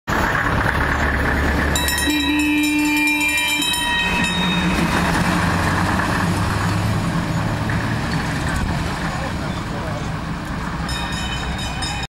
Opel-Mannschaftswagen der Feuerwehr Mylau
Opel-Mannschaftswagen der Freiwilligen Feuerwehr Mylau von 1928 bei IFA Treffen Rebesgrün / Auerbach/V. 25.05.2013.